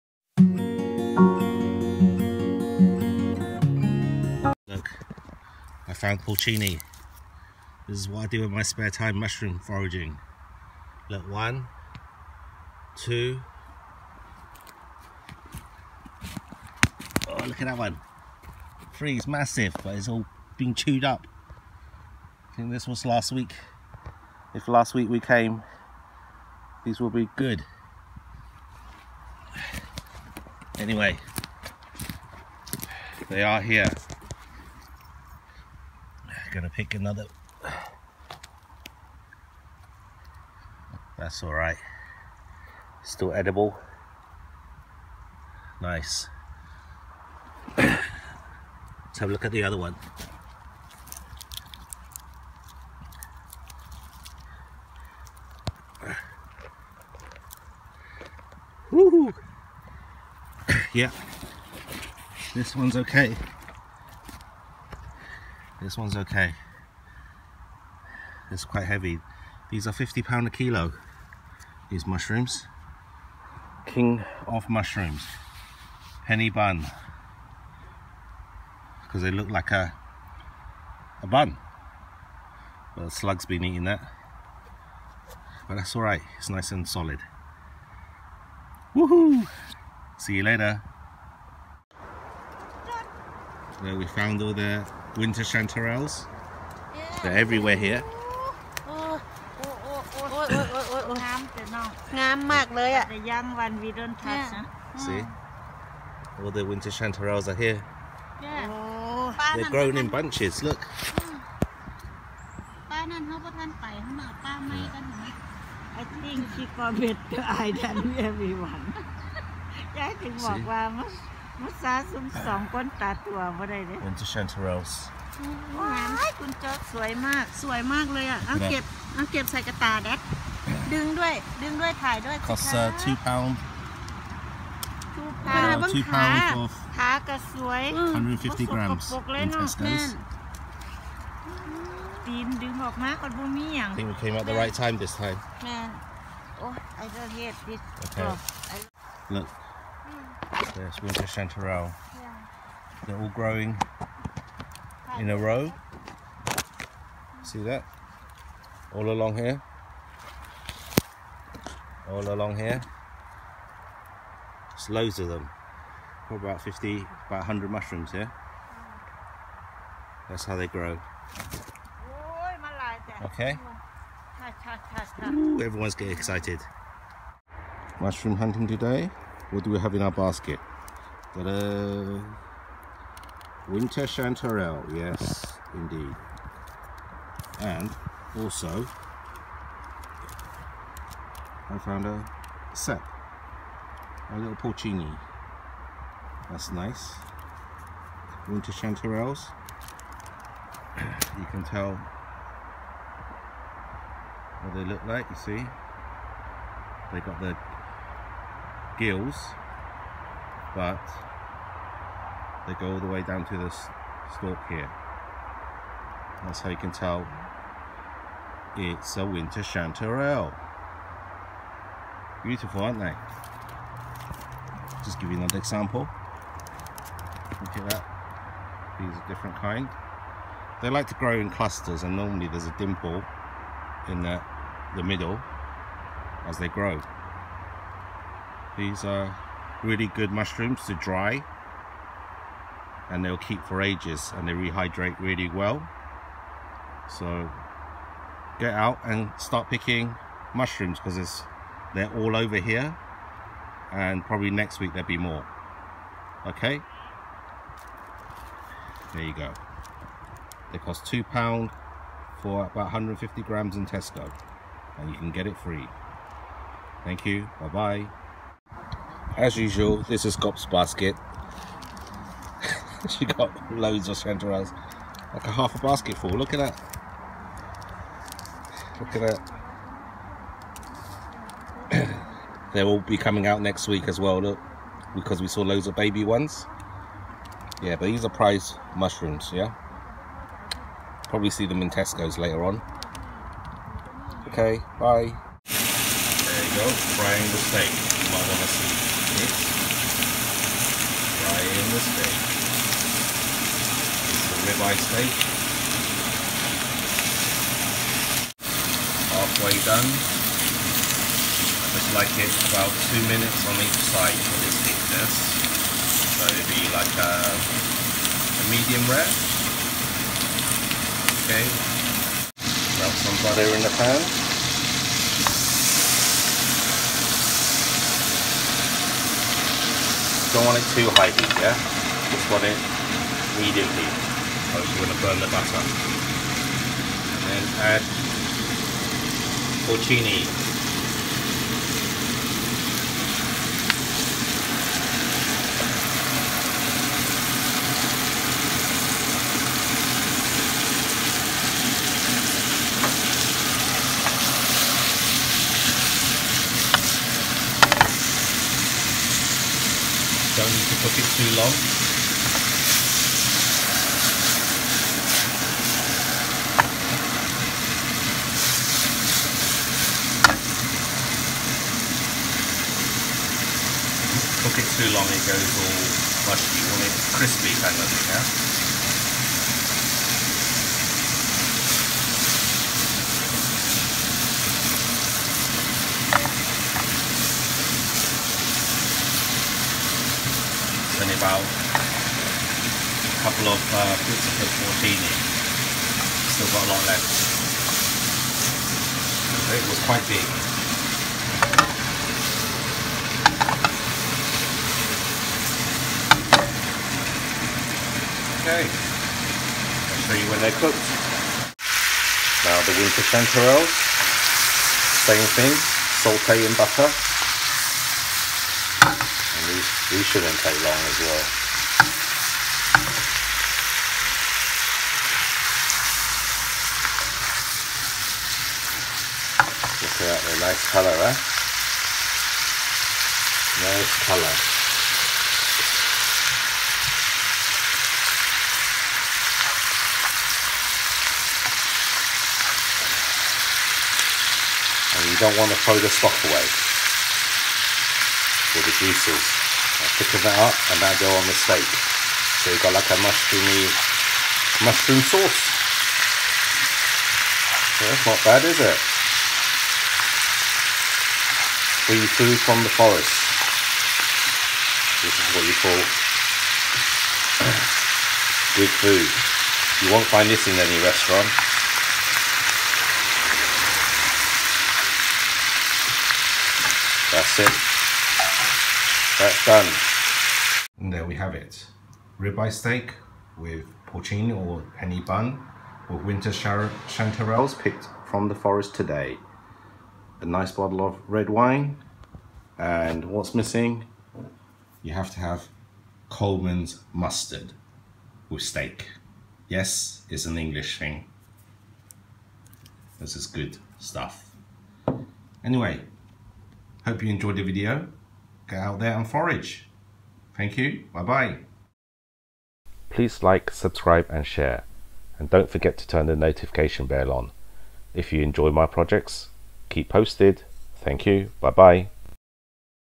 Mushroom Hunting UK. Porcini.Penny Bun. Winter Chanterelle. Foraging For Mushrooms. Cooking Mushroom
This is a video with the cooking added to the foraging of the mushrooms today.

Do not eat any mushrooms that you cannot 100% identify.

Happy eating